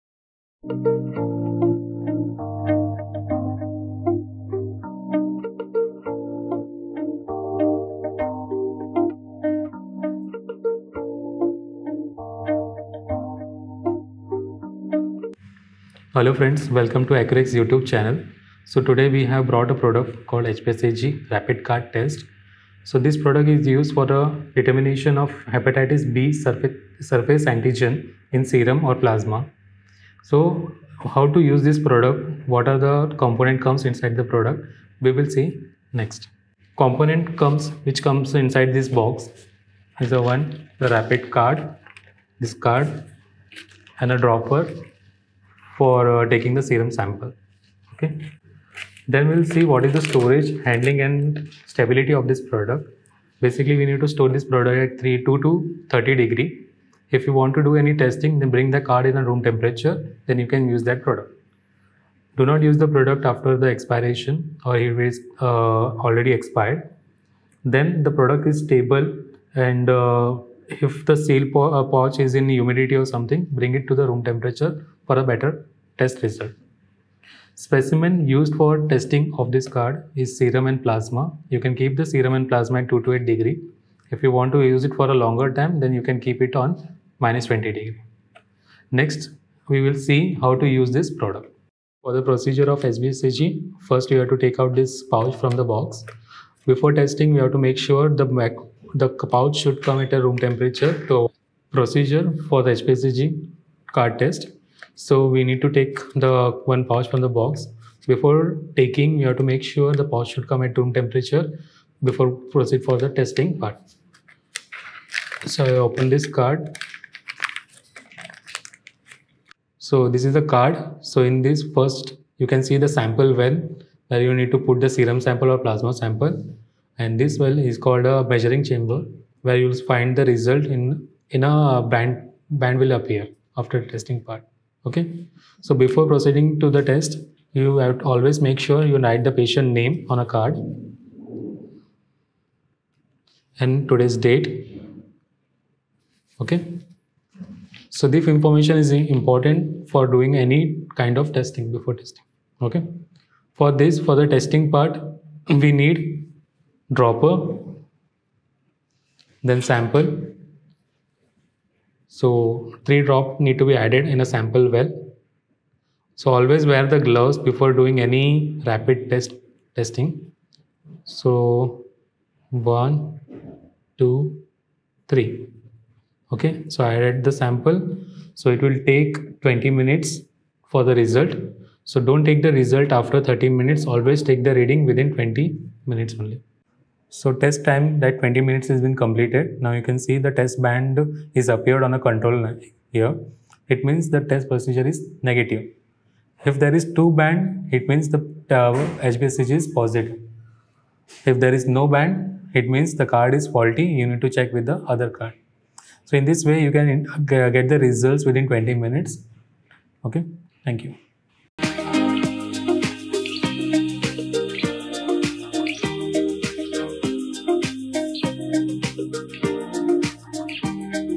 AccuTest HBsAg Rapid Test | Hepatitis B Test | HBsAg positive| Hepatitis B Surface Antigen | Accurex
HBsAg assays are routinely used to diagnose suspected HBV infection and monitor the status of infected individuals to determine whether the infection has resolved or the patient has become a chronic carrier of the virus.

1. Introduction
2. Components
3. How to proceed for this testing

1. Introduction
• Use of this kit

2. Components
• Rapid Card
• Dropper

3. Handling and Precautions
• Storage at 2° to 3°
• Don't use once it is expired
• Bring the test in the normal temperature
• Samples can be store at 2-8° or 20°

4. How to use
• Take the test pouch
• Write the patient's information on the card
• Add 3 drops of serum/plasma in sample well
• Get the result in 20 minutes